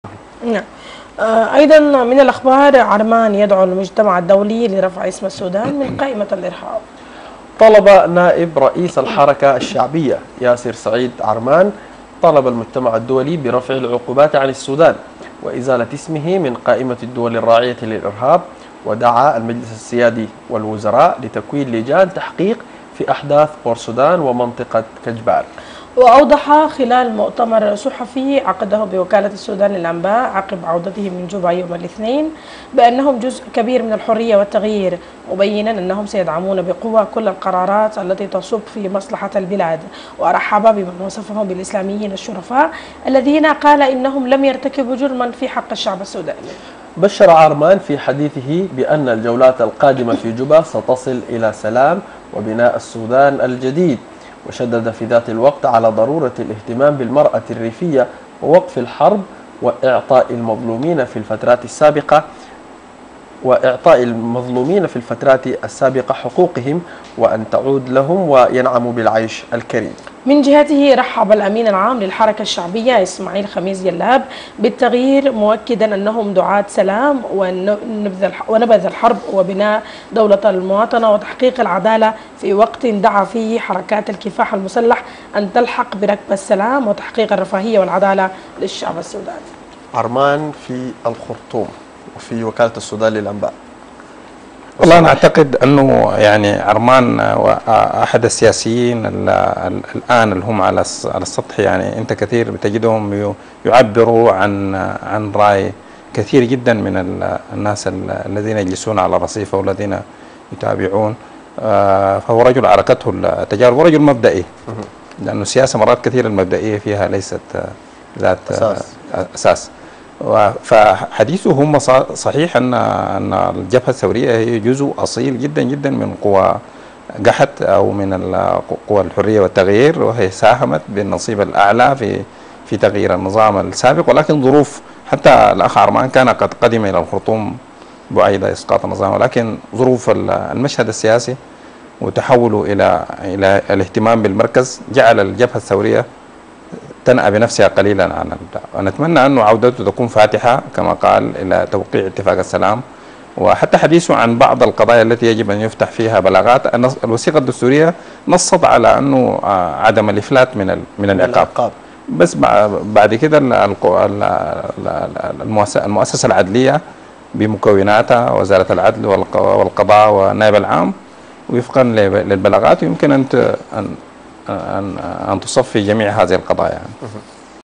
عرمان يدعو المجتمع الدولي لرفع اسم السودان من قائمة الإرهاب
الشروق نت | عرمان يدعو المجتمع الدولي لرفع اسم السودان من قائمة الإرهاب
تحليل الخبر مع رئيس تحرير شبكة الشروق نت الأستاذ/ الشيخ يوسف
ـــــــــــــــــــــــــــــــــــــــــــــــــــــــــــــ

الشروق_ الفضائية
Ashorooq Tv
ـــــــــــــــــــــــــــــــــــــــــــــــــــــــــــــ